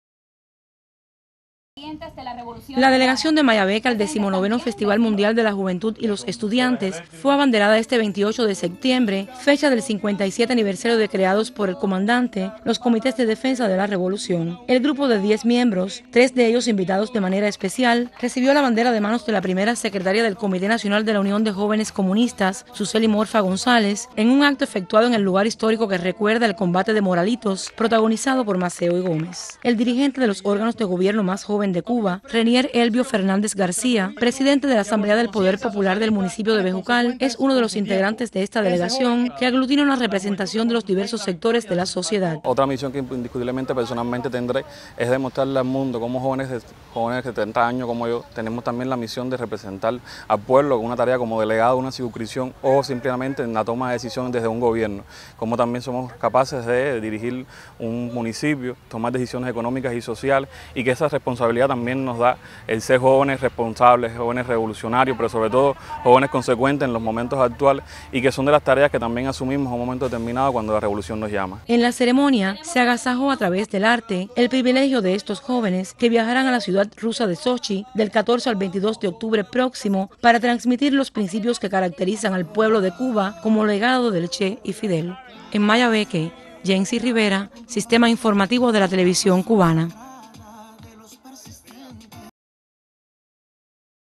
Abanderan delegación de Mayabeque al Festival mundial de la Juventud y los Estudiantes
La delegación de la provincia de Mayabeque que participará en el décimo noveno Festival Mundial de la Juventud y los Estudiantes fue abanderada en la tarde de este jueves con la presencia de la Primera secretaria del Comité Nacional de la UJC, Susely Morfa González.